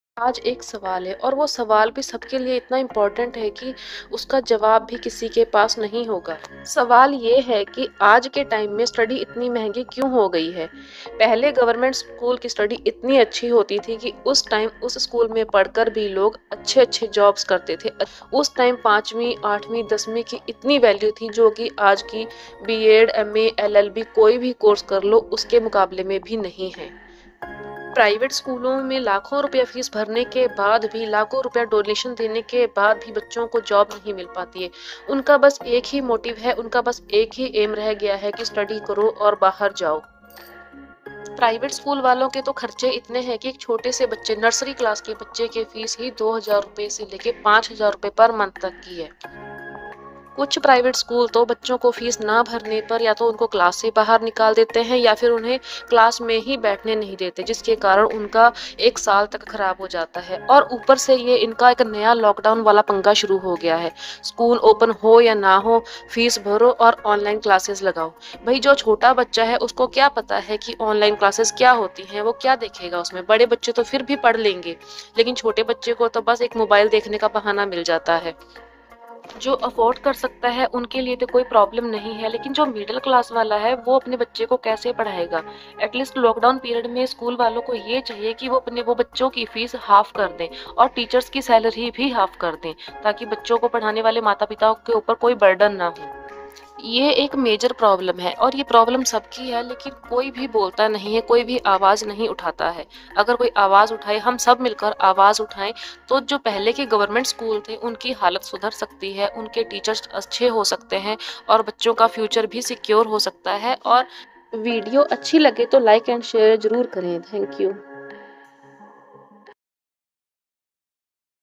`` ~Private School Ka Jinn एक प्राइवेट स्कूल के जिन्न की कहानी`
the private School Jinn was created in after the 1947 in India , Here Is all information of a private school jinn which cost was very high and jinn feature is very less in this segment.
private school ke jinn ki kahani
story of jinn
story of private school
private school ki fees
private English language school